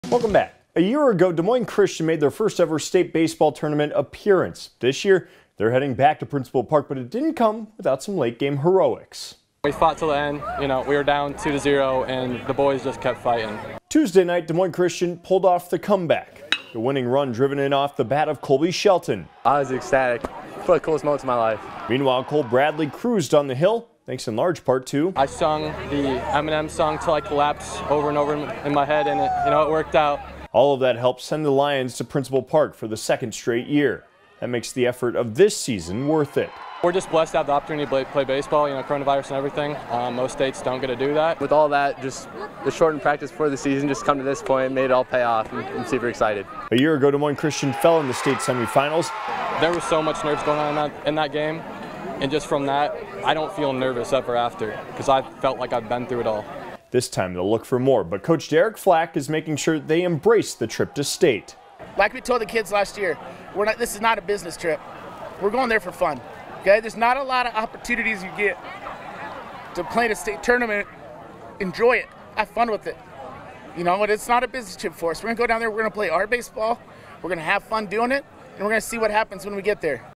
Des Moines Christian Baseball ready for return to Principal Park for state tournament
DSM Christian made their state tournament debut in 2019, now are heading back after some late game drama in their substate final.